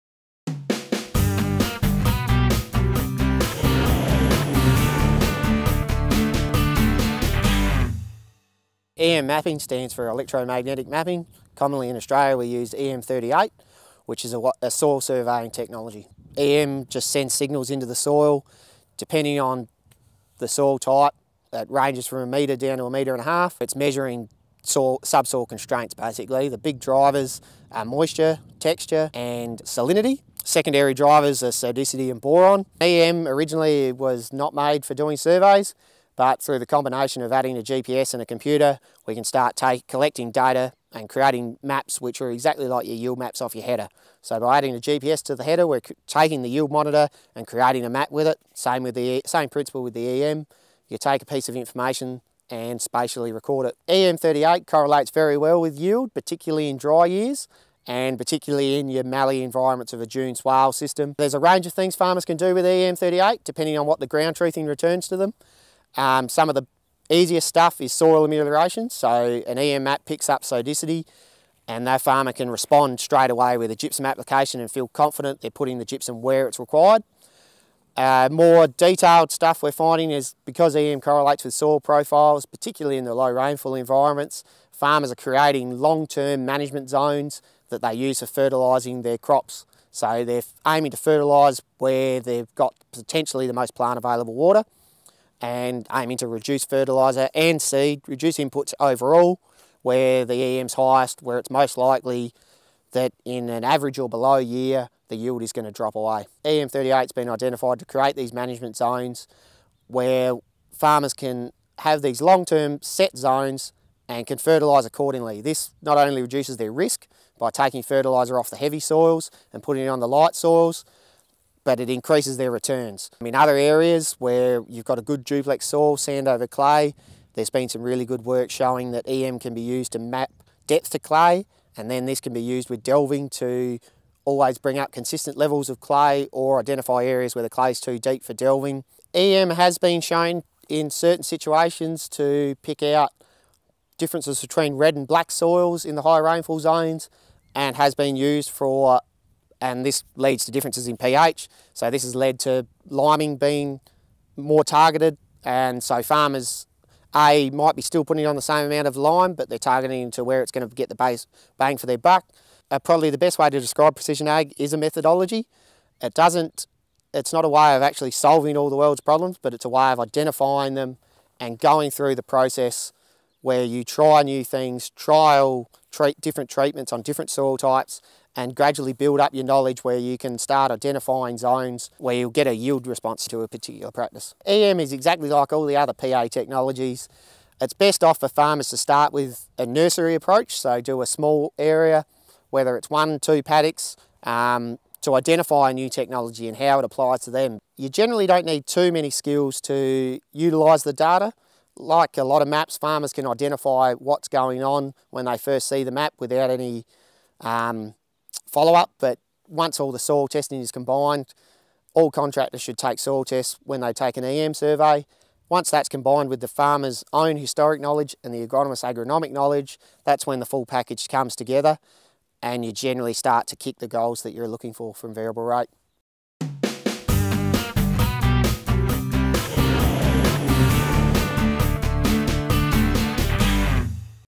Using EM for Mapping & Soil Amelioration
Peter Treloar talks about electro-magnetic (EM) mapping in precision agriculture.